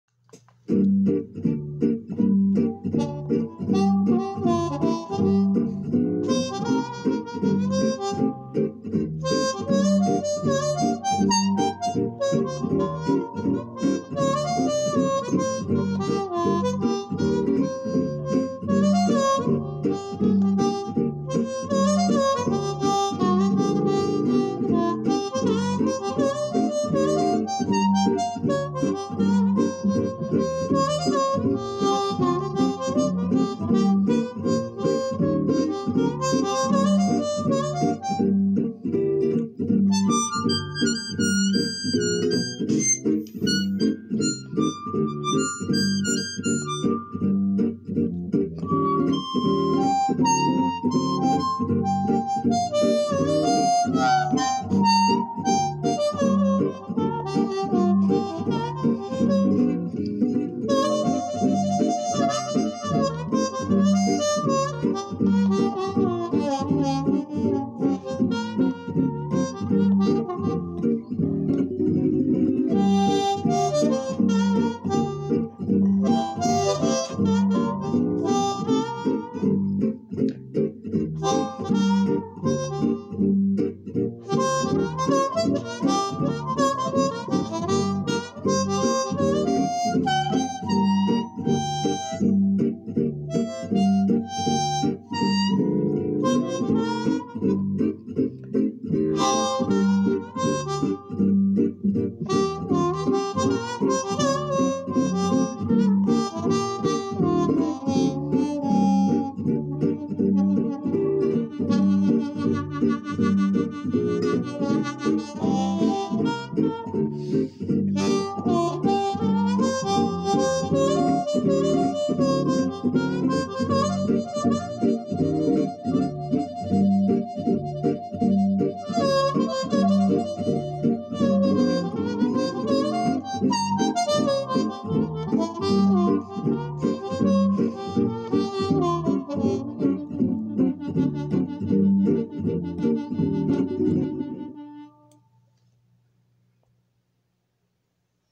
WEE VERB WAH - ROBERT TEMPLE - G MINOR GYPSY JAZZ - C HARP - 5.20.23
Some colorful riffs over G minor Gypsy Jazz. I played a C harp into a WEE VERB WAH harmonica resonator.